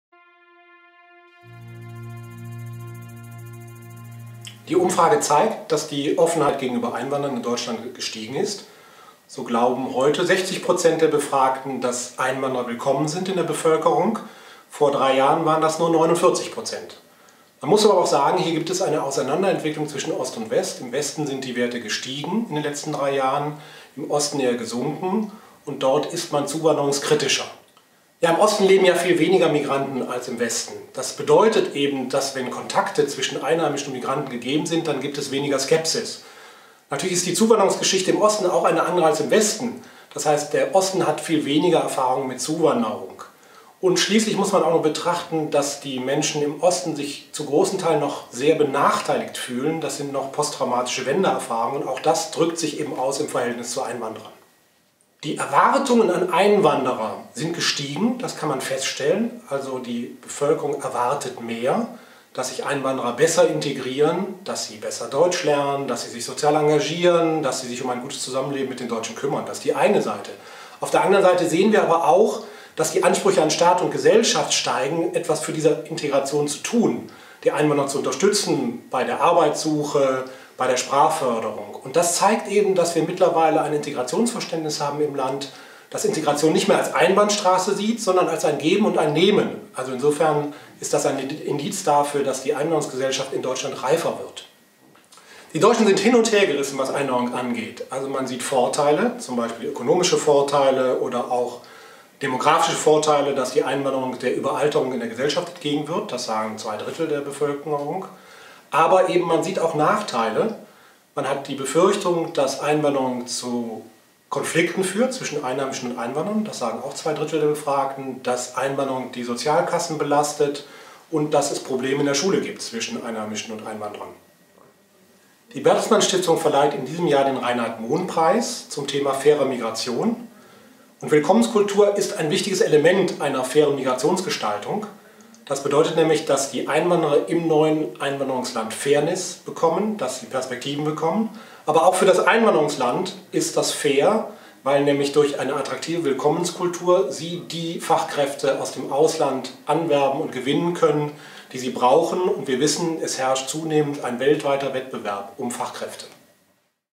Willkommenskultur in Deutschland: Entwicklungen und Herausforderungen
Deutschland ist Europas Einwanderungsland Nummer 1. Tausende kommen in der Hoffnung auf Arbeit und eine bessere Lebensperspektive. Doch nur dort, wo sich Menschen wohlfühlen, bleiben sie dauerhaft. Wie aber sieht die aktuelle Haltung der Deutschen gegenüber Einwanderern aus? Unsere Umfrage gibt Antworten.

Ergebnisse einer repräsentativen Bevölkerungsumfrage in Deutschland - Einschätzungen von Ulrich Kober, Programmleiter Integration und Vielfalt Bertelsmann Stiftung